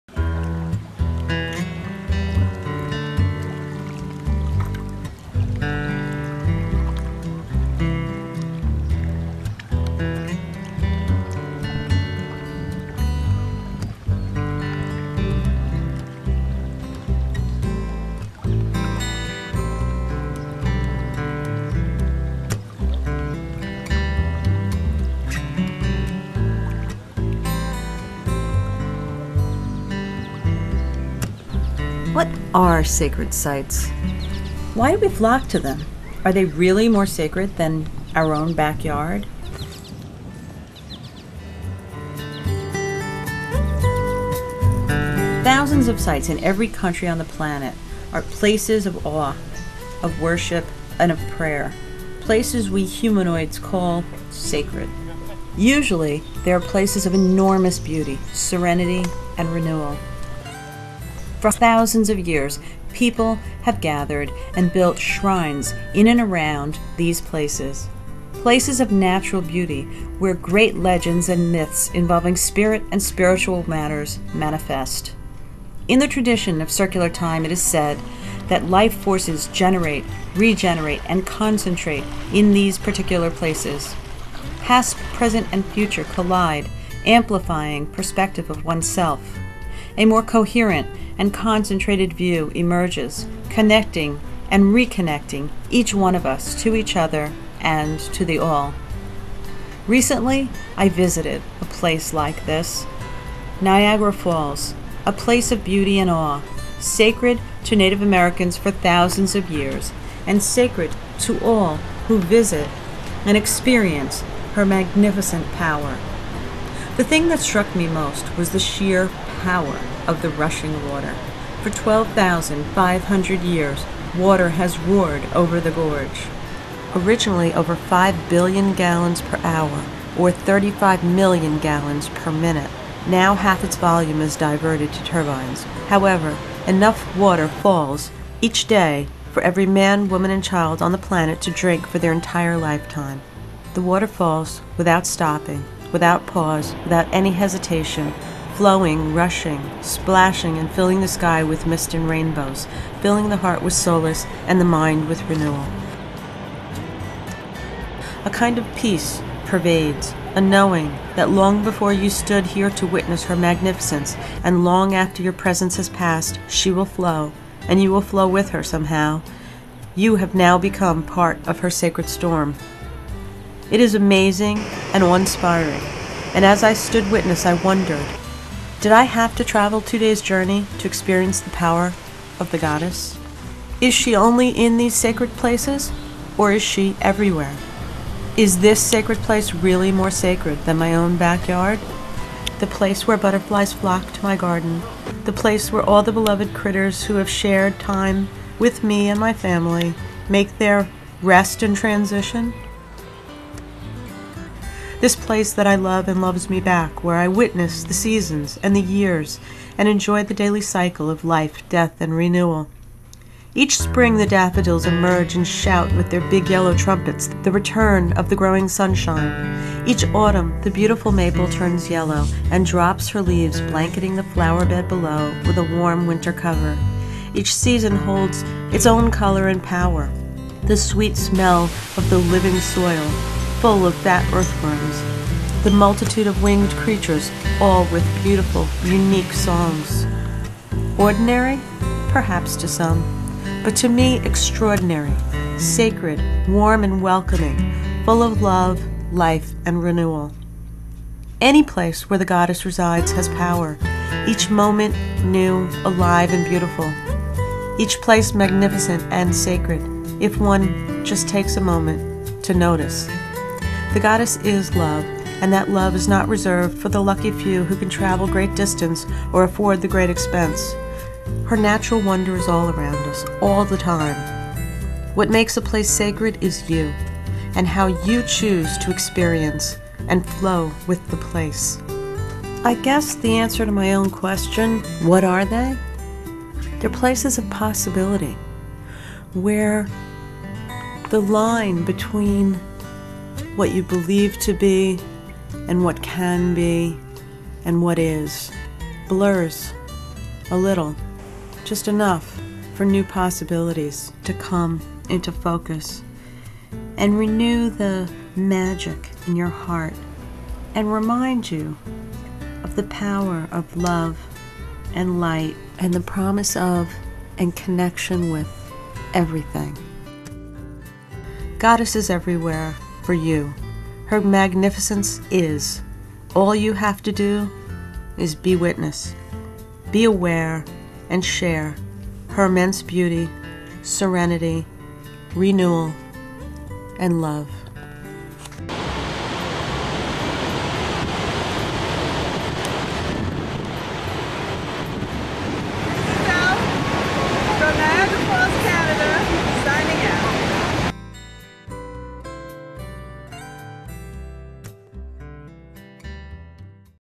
Sacred Sites
What are sacred sites? Are they really more sacred than our own backyard?Places of possibility... where the line between what you believe to be and what can be and what is blurs a little, just enough for new possibilities to come into focus.